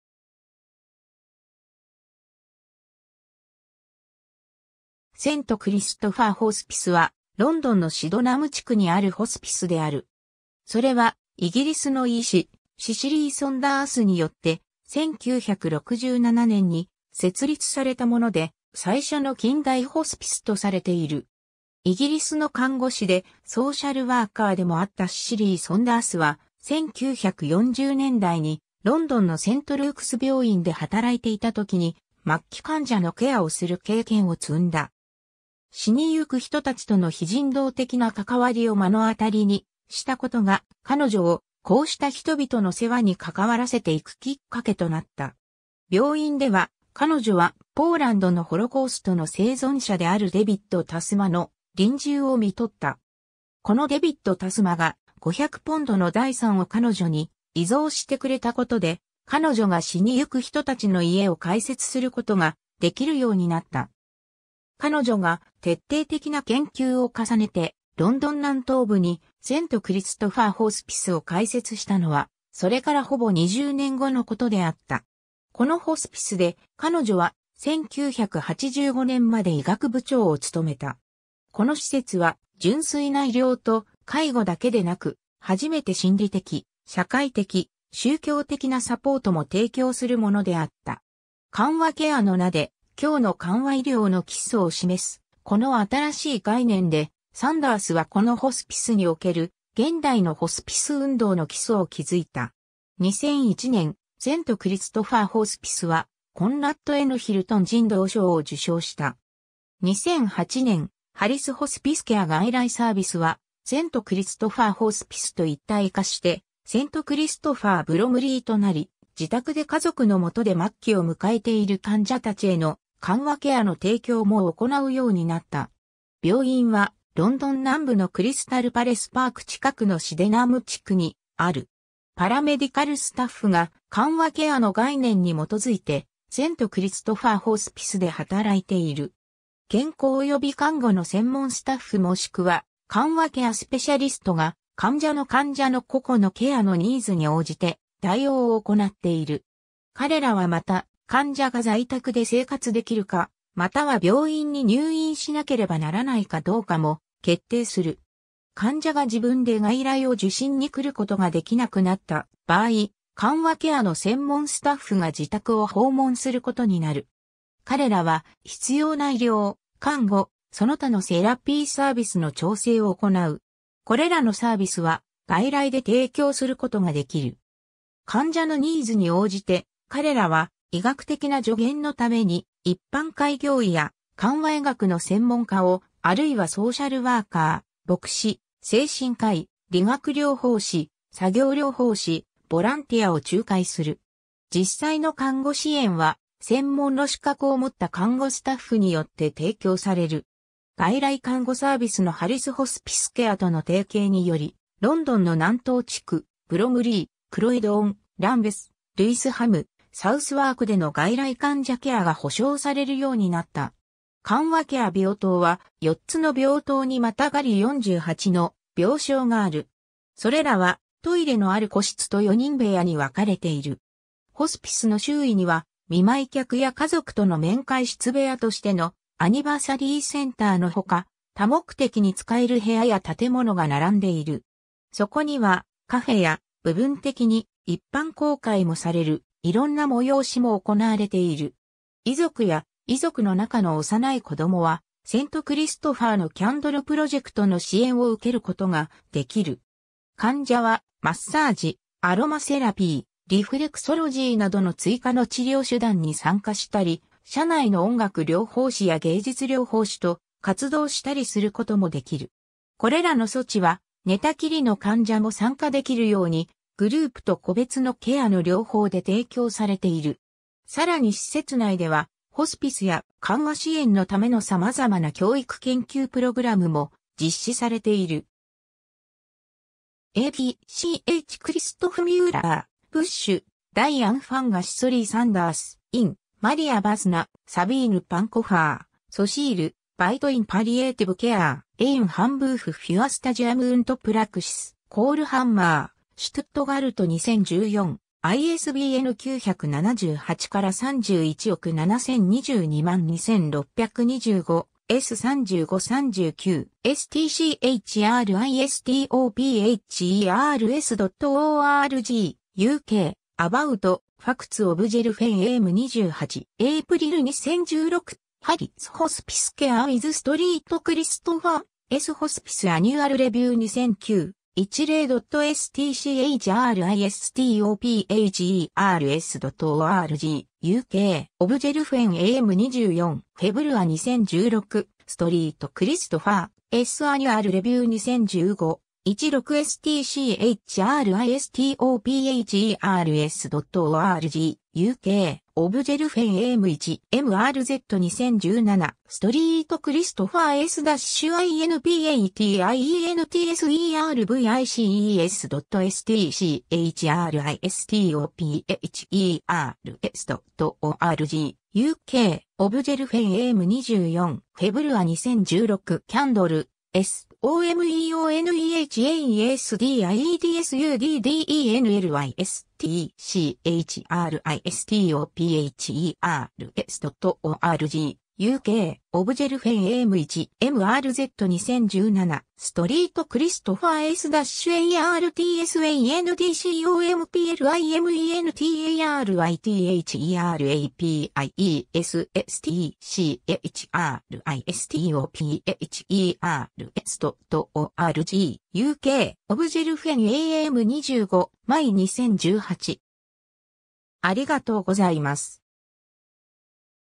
セント・クリストファー・ホスピス
セント・クリストファー・ホスピス(St Christopher’s Hospice)は、ロンドンのシドナム地区にあるホスピスである。
それは、イギリスの医師、シシリー・ソンダースによって、1967年に設立されたもので、最初の近代ホスピスとされている。
 イギリスの看護師でソーシャルワーカーでもあったシシリー・ソンダースは、1940年代にロンドンのセント・ルークス病院で働いていたときに、末期患者のケアをする経験を積んだ。
 死にゆく人たちとの非人道的な関わりを目の当たりにしたことが、彼女をこうした人々の世話に関わらせていくきっかけとなった。
 病院では、彼女はポーランドのホロコーストの生存者であるデビッド・タスマの臨終を看取った。
このデビッド・タスマが、500ポンドの財産を彼女に遺贈してくれたことで、彼女が死にゆく人たちの家を開設することができるようになった。
 彼女が、徹底的な研究を重ねて、ロンドン南東部にセント・クリストファー・ホスピスを開設したのは、それからほぼ20年後のことであった。
このホスピスで、彼女は、1985年まで医学部長を勤めた。
この施設は、純粋な医療と介護だけでなく、初めて心理的、社会的、宗教的なサポートも提供するものであった。
緩和ケア(Palliative Care)の名で、今日の緩和医療の基礎を示すこの新しい概念で、サンダースはこのホスピスにおける現代のホスピス運動の基礎を築いた。
 2001年、セント・クリストファー・ホスピスはコンラッド・N・ヒルトン人道賞を受賞した。
 2008年、「ハリス・ホスピスケア」外来サービスはセント・クリストファー・ホスピスと一体化して、「セント・クリストファー・ブロムリー」となり、自宅で家族のもとで末期を迎えている患者たちへの緩和ケアの提供も行うようになった.。
 病院は、ロンドン南部のクリスタル・パレス・パーク近くのシデナム地区にある。
 パラメディカルスタッフが、緩和ケアの概念に基づいてセント・クリストファー・ホスピスで働いている。
健康および看護の専門スタッフ（臨床看護スペシャリスト（CNS）もしくは緩和ケアスペシャリスト）が、患者の患者の個々のケアのニーズに応じて対応を行っている。
彼らはまた、患者が在宅で生活できるか、または病院に入院しなければならないかどうかも決定する。
患者が自分で外来を受診に来ることができなくなった場合、緩和ケアの専門スタッフが自宅を訪問することになる。
彼らは必要な医療、看護、その他のセラピーサービスの調整を行う。
これらのサービスは外来で提供することができる。
 患者のニーズに応じて、彼らは、医学的な助言のために一般開業医や緩和医学の専門家を、あるいはソーシャルワーカー、牧師、精神科医、理学療法士、作業療法士、ボランティアを仲介する。
実際の看護支援は、専門の資格を持った看護スタッフによって提供される。
 外来看護サービスのハリス・ホスピスケアとの提携により、ロンドンの南東地区、ブロムリー、クロイドン、ランベス、ルイスハム、サウスワークでの外来患者ケアが保証されるようになった。
 緩和ケア病棟(ホスピス)は、4つの病棟にまたがり48の病床がある。
それらはトイレのある個室と４人部屋に分かれている。
ホスピスの周囲には、見舞客や家族との面会室部屋としての「アニバーサリーセンター」のほか多目的に使える部屋や建物が並んでいる。
 そこにはカフェや部分的に一般公開もされるいろんな催しも行われている。
 遺族や遺族の中の幼い子どもは、「セント・クリストファーのキャンドル・プロジェクト」の支援を受けることができる。
 患者は、マッサージ、アロマセラピー、リフレクソロジーなどの追加の治療手段に参加したり、社内の音楽療法士や芸術療法士と活動したりすることもできる。
これらの措置は、寝たきりの患者も参加できるように、グループと個別のケアの両方で提供されている。
さらに施設内では、ホスピスや緩和支援のためのさまざまな教育研究プログラムも実施されている。
 ^ a b c H. Christof Müller-Busch: Die Anfänge - Cicely Saunders. In: Maria Wasner, Sabine Pankofer (Hrsg.): Soziale Arbeit in Palliative Care. Ein Handbuch für Studiu...